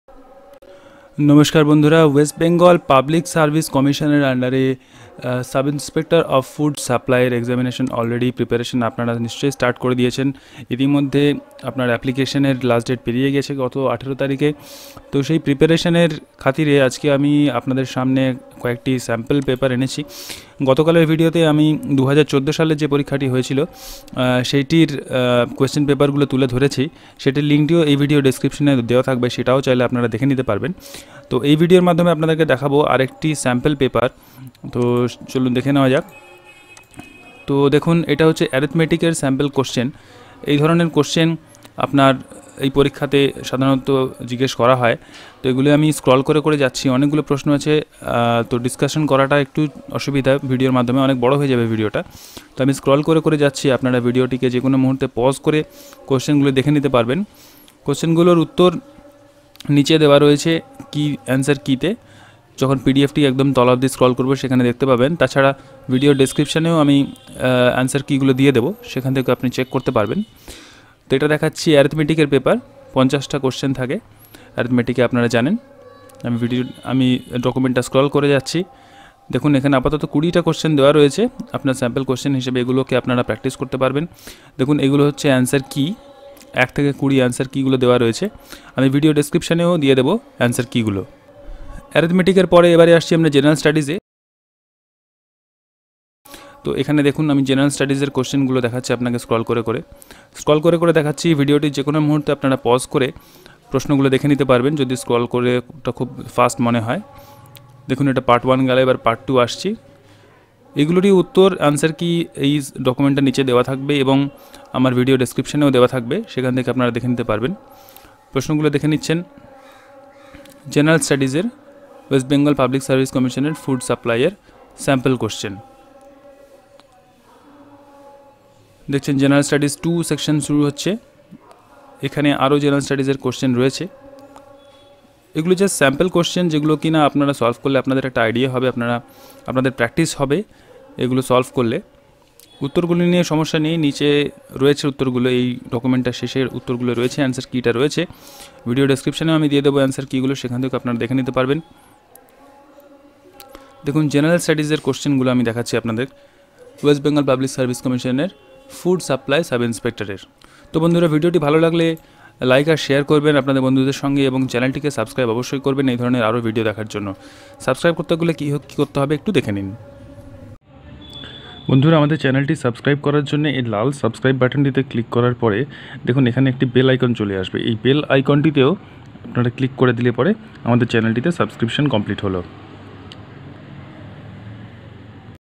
WBPSC Food Supply Sub Inspector 2018 Sample Questions FOODSUPPLY SUB INSPECTOR Exam Watch
Exam Watch welcomes you

Answer Key to Sample Paper WB Food Supply Sub Inspector (Arithmetic):
1. B
2. A
3. C
4. C
5. D
6. A
7. B
8. C
9. C
10. C
11. C
12. C
13. D
14. B
15. C
16. B
17. B
18. B
19. C
20. D